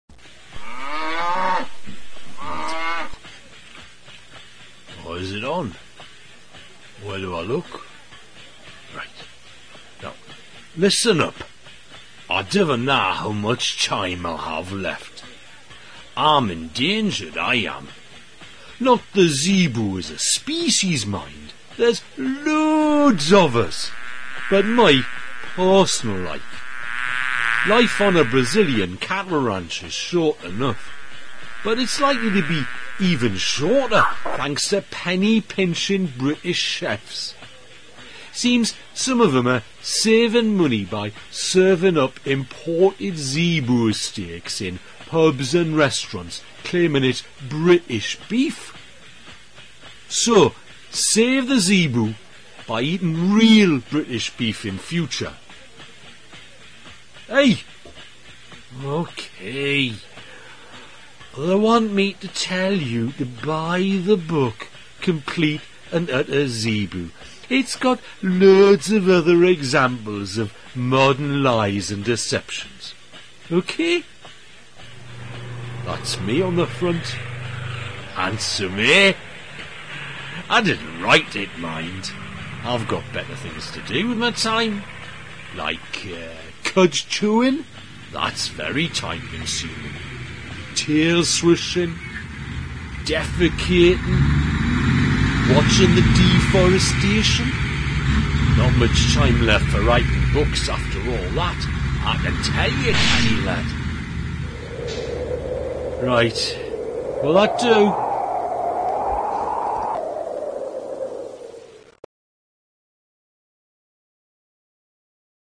Complete and Utter Zebu
Why is everyone lying to us? Complete & Utter Zebu is a cocktail of humour, outrage and indignation. Join us to discuss the zebu we're force-fed every day

Zebu (n). 1. A grotesque South American mammal whose tough meat is often passed off as 'British steak' in pubs and restaurants.
2. Lies, deception, bullsh!t.

We are subjected to a daily barrage of deception so pervasive that we barely notice it any more. Although the big lies still make the headlines, it's the small, everyday half-truths that really drive many of us to distraction. 'Ice cream' with no cream, 'chocolate flavour' with no chocolate, 'customer service' with no service. You have won a prize . . . Your call is important to us . . . Lies, lies and more damned lies. Stop it! Stop it! Stop it!

With a combination of determined research, an eye for a good story and a wicked sense of humour, bestselling authors Simon Rose and Steve Caplin expose the careless, shameless porkies told by estate agents, insurance companies, manufacturers, politicians, policemen, photographers, airlines, retailers, restaurants and advertisers. It's a miscellany of misinformation to entertain and horrify, amuse and outrage.